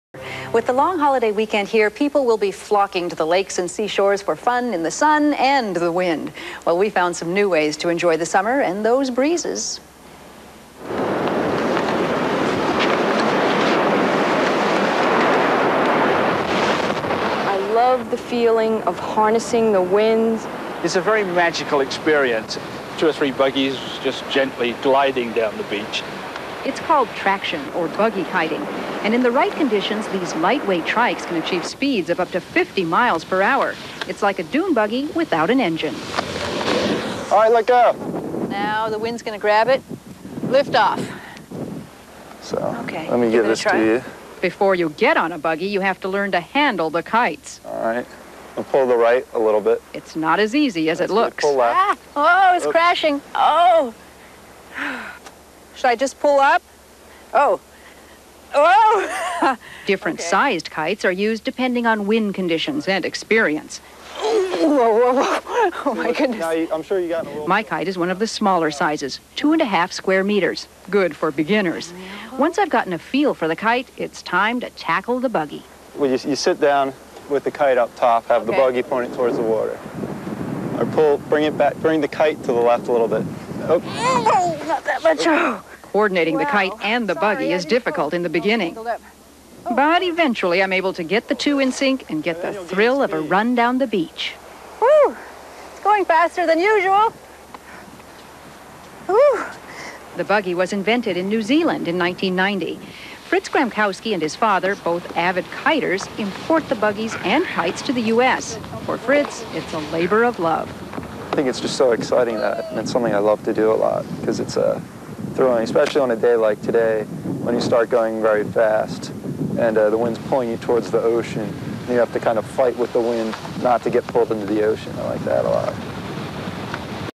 Kite Buggy
Fritz Gramkowski demonstrates traction kiting with buggies for CBS morning TV.